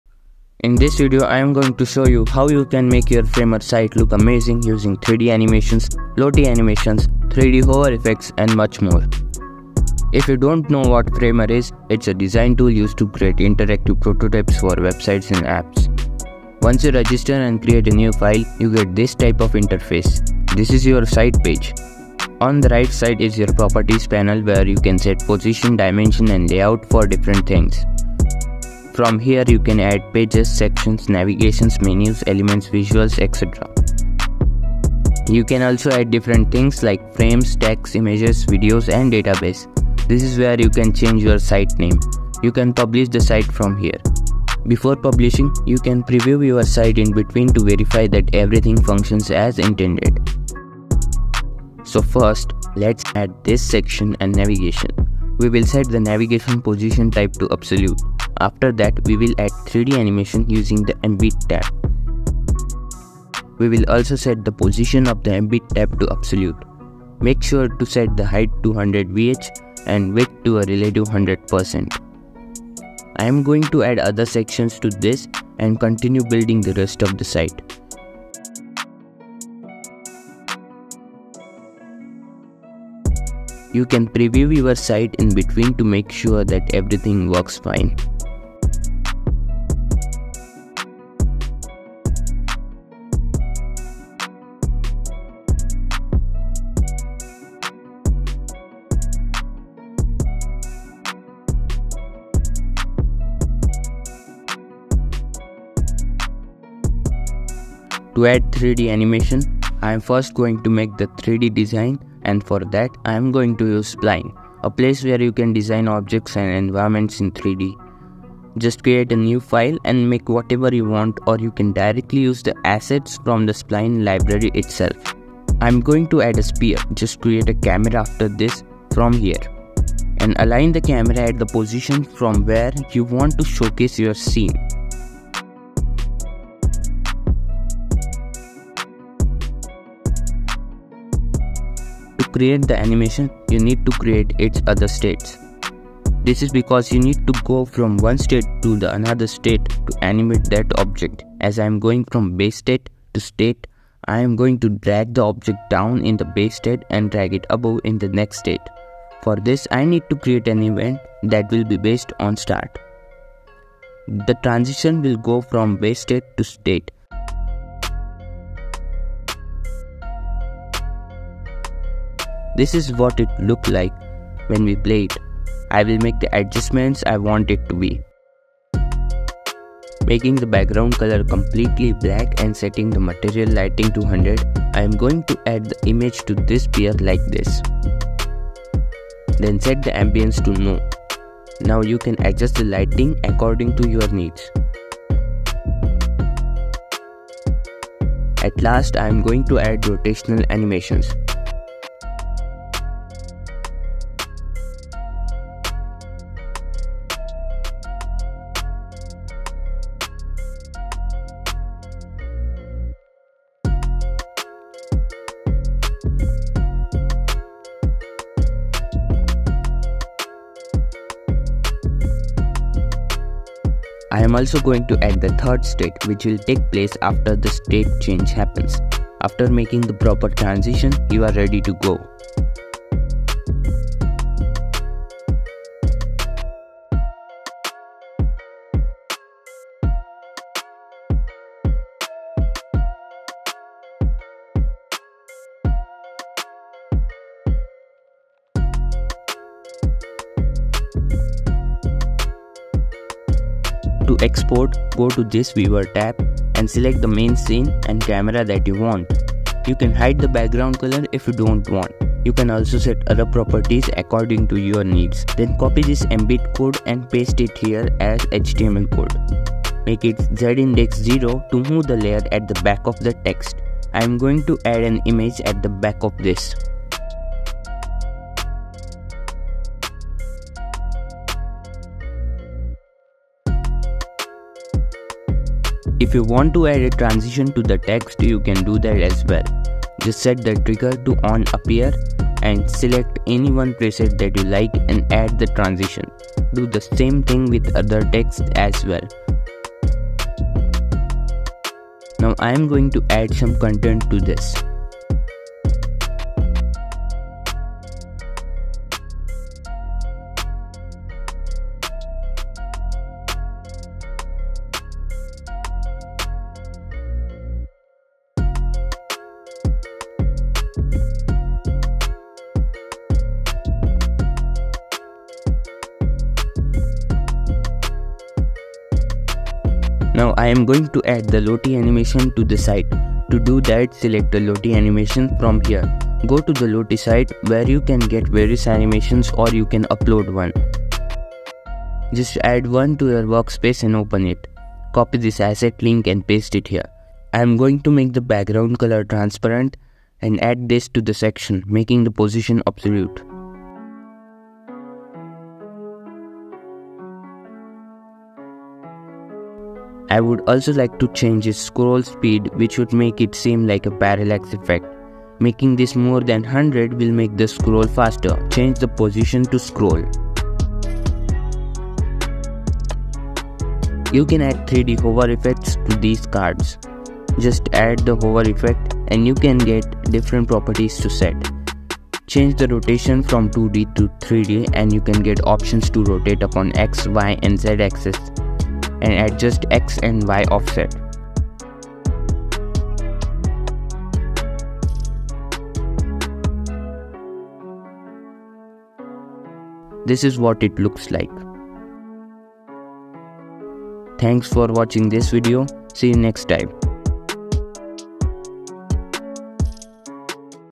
Make Your WEBSITE Stand Out in 2024 With 3D Animations, Lottie Animations and Amazing Hover Effects
This video is a tutorial about creating a website with interactive elements using Framer, a design tool. Here are the key points covered in the video:

Framer overview: Framer is a tool used to design websites and apps. It allows you to create interactive prototypes, which are basically simulated versions of the final product.
3D animation with Spline: The video demonstrates how to design a 3D animation using Spline, a separate 3D design tool. The animation is then embedded into the Framer website.
Lottie animation: The tutorial also shows how to add a Lottie animation, which is a type of animation format, to the website.
3D hover effects: You can create interactive elements where objects react when the user hovers over them with the mouse.

Overall, the video offers a brief introduction to using Framer to create visually appealing and interactive websites.